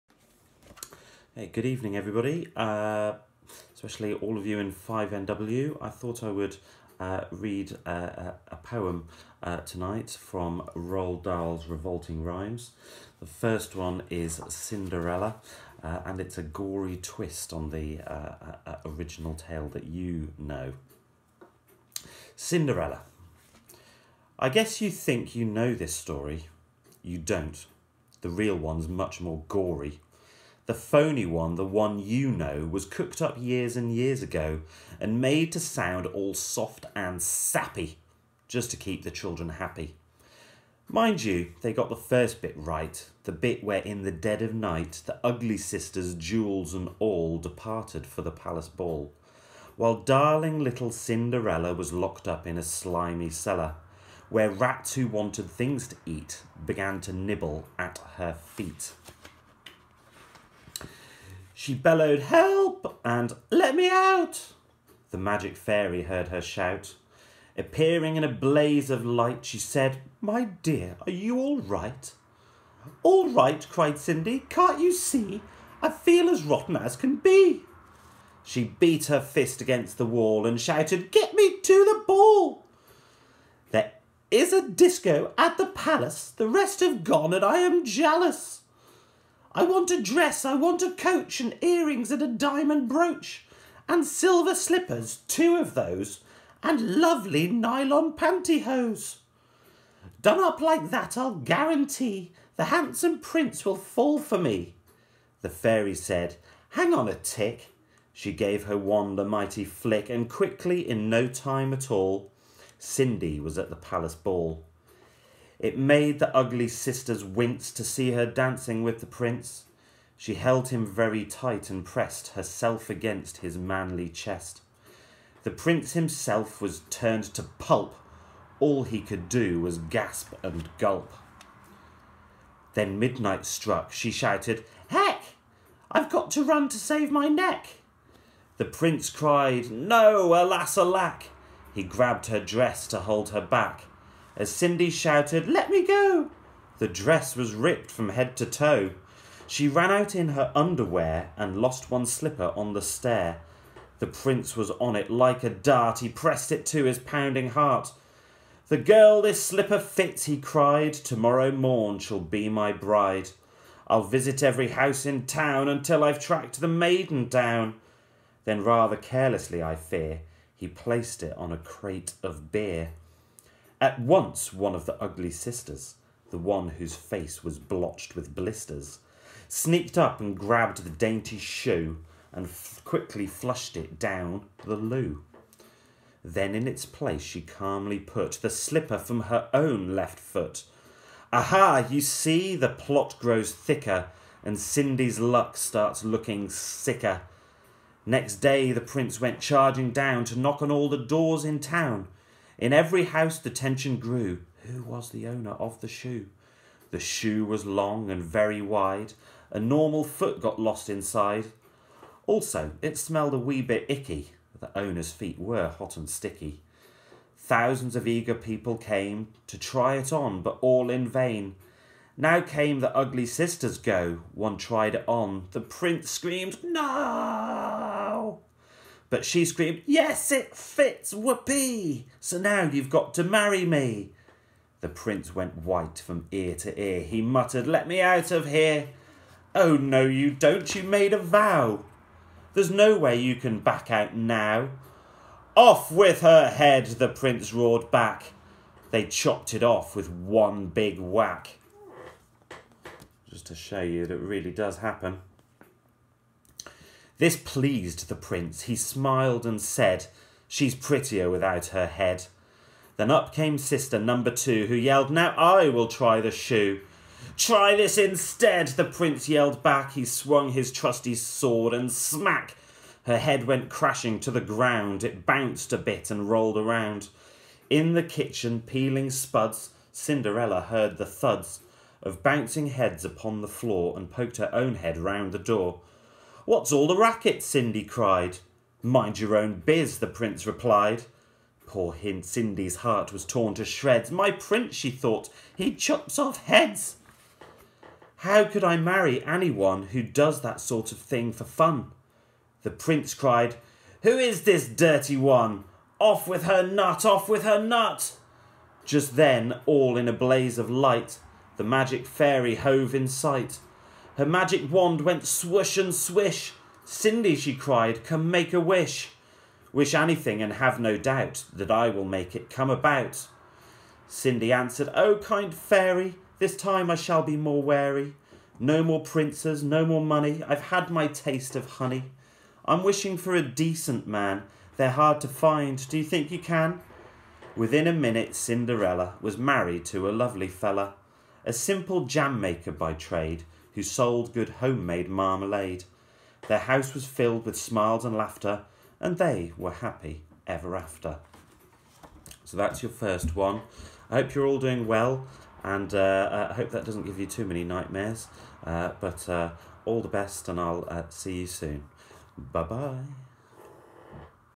Mr W Reads 'Cinderella' from Roald Dahl's Revolting Rhymes
Revolting Rhymes - Cinderella
by Roald Dahl